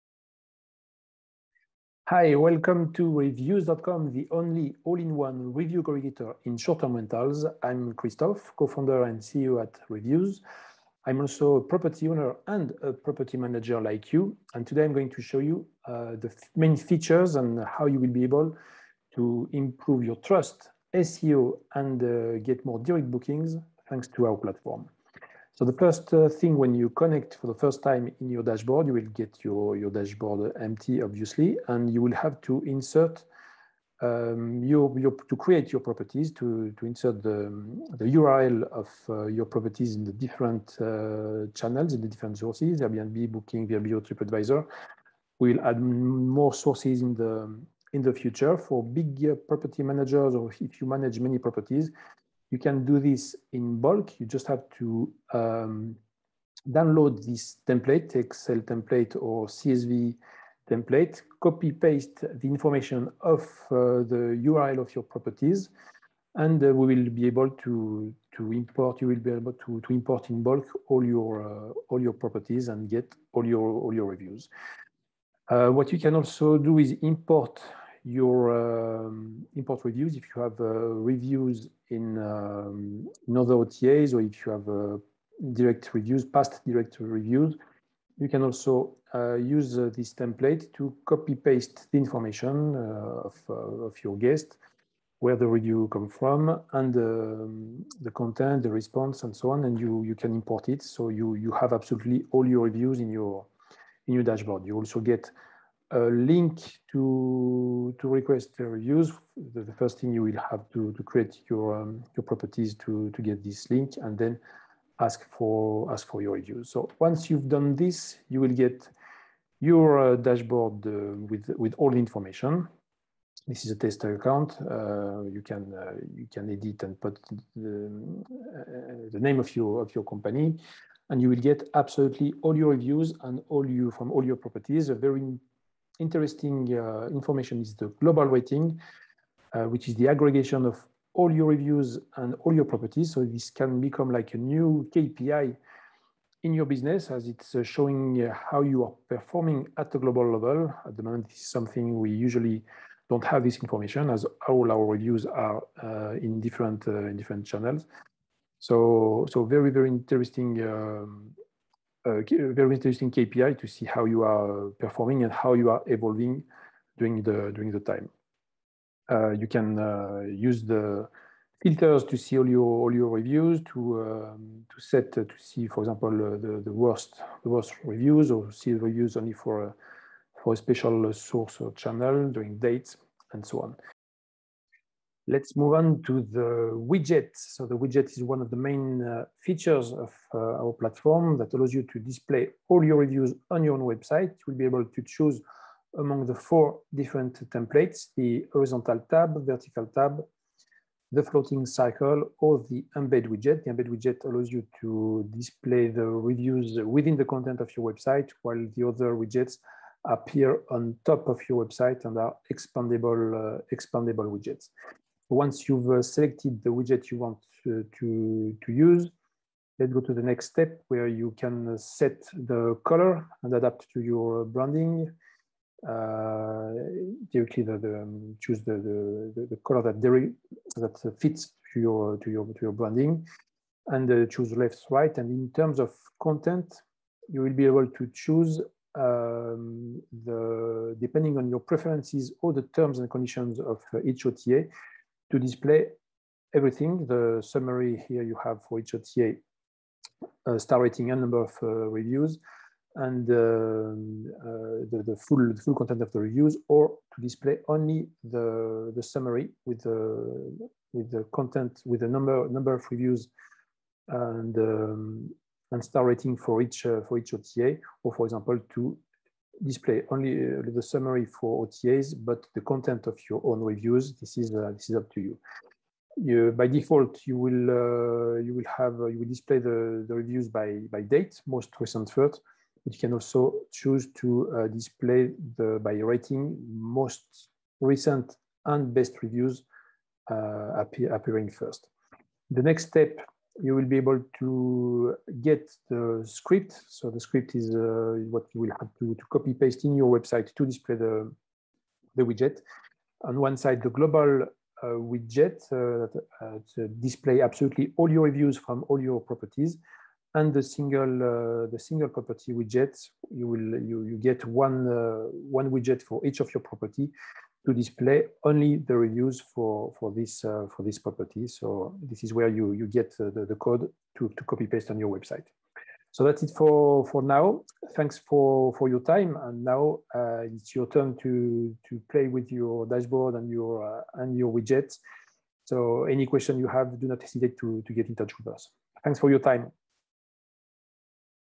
Tutorial Revyoos
In this video you will see how to setup your Revyoos account to automatically get all your reviews.